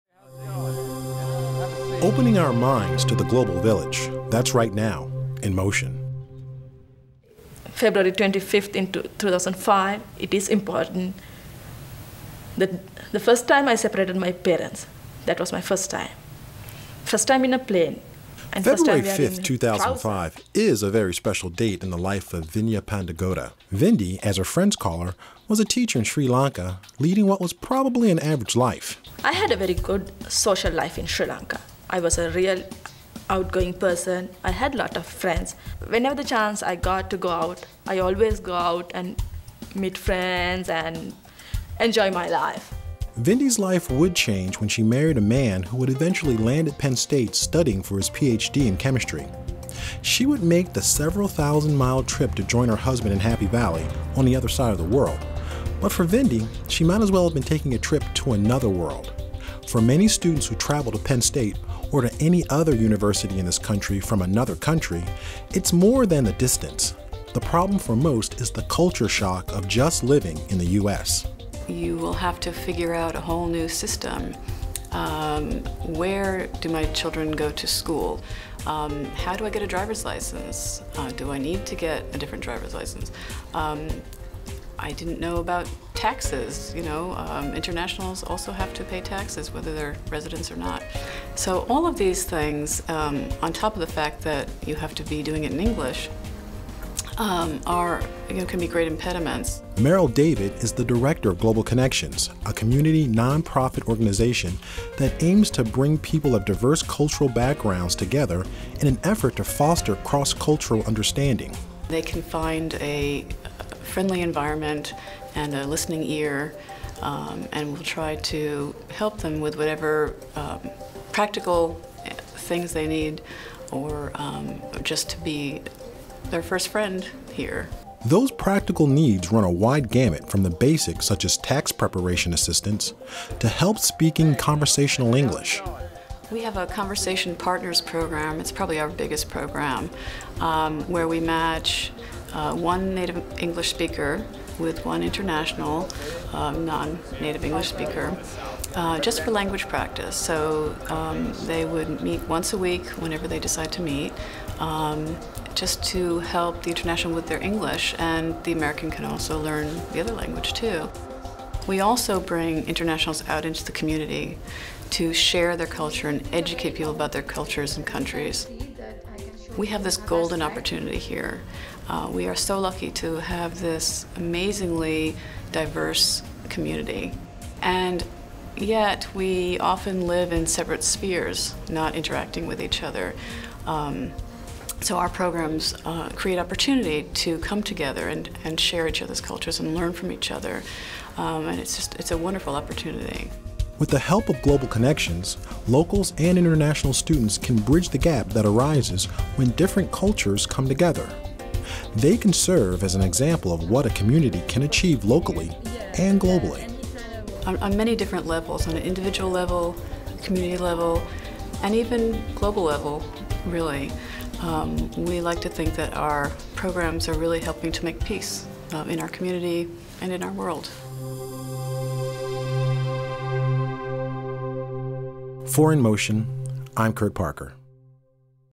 Seeking understanding with diverse populations
University Park PA- There are more than 40,000  students at the University Park campus who come from across the United States and the world to get a Penn State degree. The number of international students has grown significantly in recent years,  especially among undergraduate students, a group that in 2008 surpassed 4,000 for the first time.

When two or more populations come together sometimes a gap can arise from lack of understanding by all parties. Merrill David is the Director of Global Connections, a community non profit organization that aims to bring people of diverse cultural backgrounds together in an effort to foster cross cultural understanding. With the help of this organization international students have a resource to draw upon to help alleviate the stress of learning a new culture and locals gain a unique opportunity to learn about people from different parts of the world. For more information about programs that promote cross cultural sharing at Penn State . Produced by: C Roy Parker